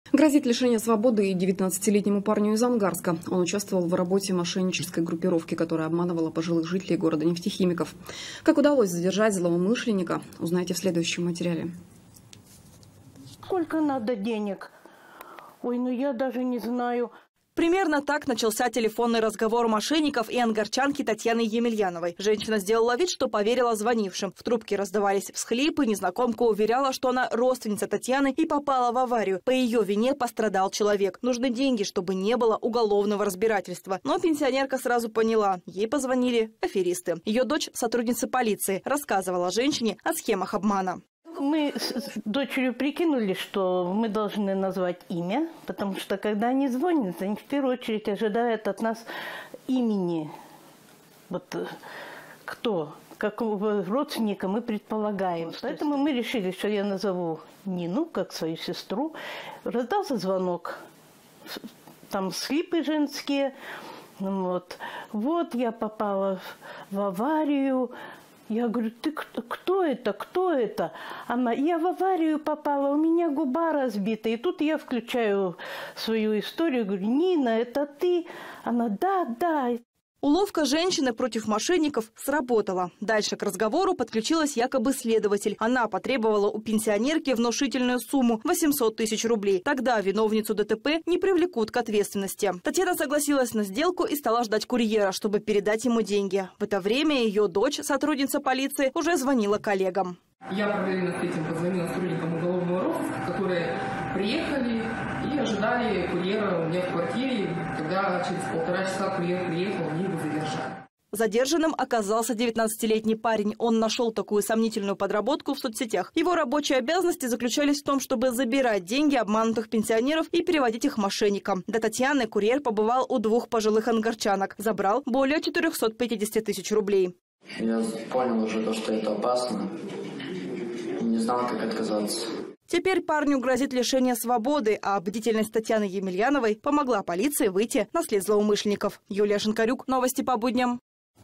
Пенсионерка из Ангарска дала отпор телефонным мошенникам
Грозит лишение свободы 19-летнему парню из Ангарска. Он участвовал в работе мошеннической группировки, которая обманывала пожилых жителей города нефтехимиков. Как удалось задержать злоумышленника узнаете в следующем материале.

— А сколько надо денег? Ой, ну я даже не знаю...

Примерно так начался телефонный разговор мошенников и ангарчанки Татьяны Емельяновой. Женщина сделала вид, что поверила звонившим. В трубке раздавались всхлипы, незнакомка уверяла, что она – родственница Татьяны и попала в аварию. По её вине пострадал человек. Нужны деньги, чтобы не было уголовного разбирательства. Но пенсионерка сразу поняла, что ей позвонили аферисты. Её дочь, сотрудник полиции, рассказывала женщине о схемах обмана.

ТАТЬЯНА ЕМЕЛЬЯНОВА, жительница Ангарска:

«Мы с дочерью прикинули, что должны сами назвать имя. Потому что когда они звонят, они в первую очередь ожидают, что мы назовём имя родственника. Поэтому мы решили, что я назову имя "Нина", как у моей сестры. Раздался звонок, там всхлипы женские: "Вот, я попала в аварию". Я говорю: "Кто это?" Она отвечает: "Я в аварию попала, у меня губа разбита". И тут я включаю свою историю и говорю: "Нина, это ты?". Она отвечает: "Да"».

Уловка женщины против мошенников сработала. Дальше к разговору подключилась якобы следователь. Она потребовала у пенсионерки внушительную сумму – 800 тысяч рублей. Тогда виновницу ДТП не привлекут к ответственности. Татьяна согласилась на сделку и стала ждать курьера, чтобы передать ему деньги. В это время её дочь, сотрудница полиции, уже звонила коллегам.

ЕЛЕНА ЗОДБОЕВА, старший оперативный дежурный УМВД России по Ангарскому округу:

«Я параллельно с этим позвонила сотрудникам уголовного розыска, которые приехали и ожидали курьера уже в квартире, когда он через полтора часа приехал, его задержали».

Задержанным оказался 19-летний парень. Его рабочие обязанности заключались в том, чтобы забирать деньги обманутых пенсионеров и переводить их мошенникам. До Татьяны курьер забрал у двух пожилых ангарчанок более 450 тысяч.

— Я понял, что уже это опасно, но не знал уже, как отказаться.

Теперь парню грозит лишение свободы. А бдительность Татьяны Емельяновой помогла полиции выйти на след злоумышленников.